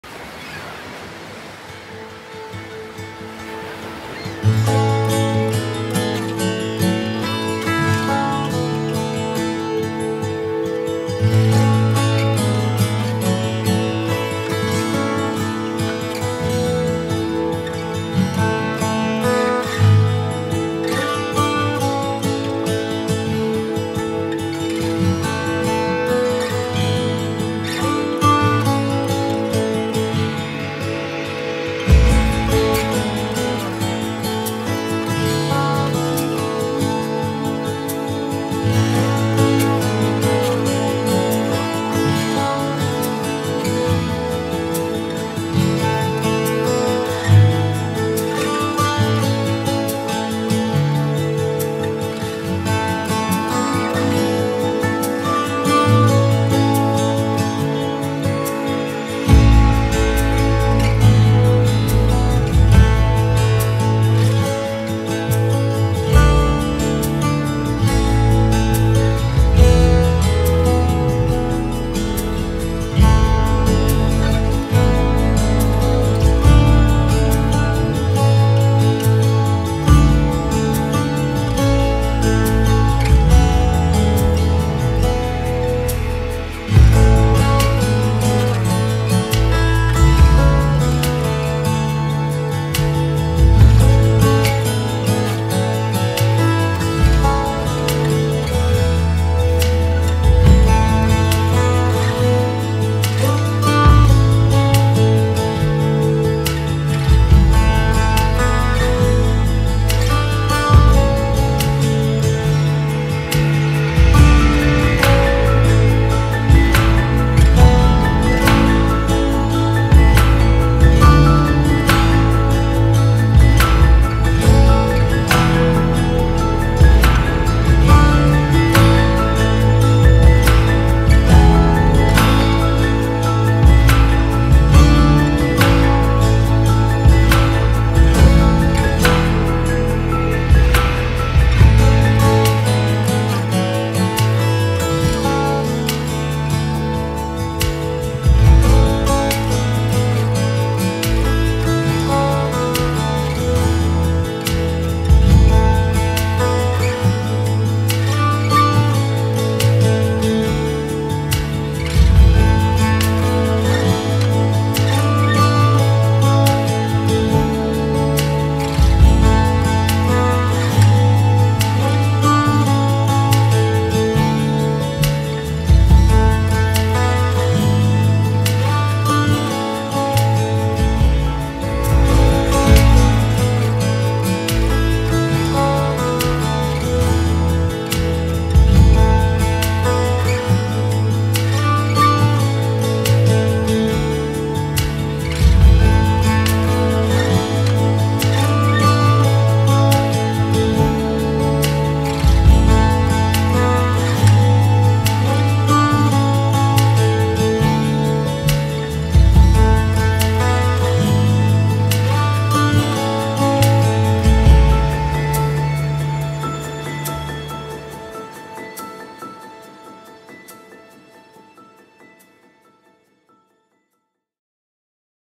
26 Crescent Drive, Brielle, NJ
Paradise. a boater's dream come true. a rare opportunity to own this exceptional home on almost an acre of deep waterfront property with unobstructed quick access to this beautiful home.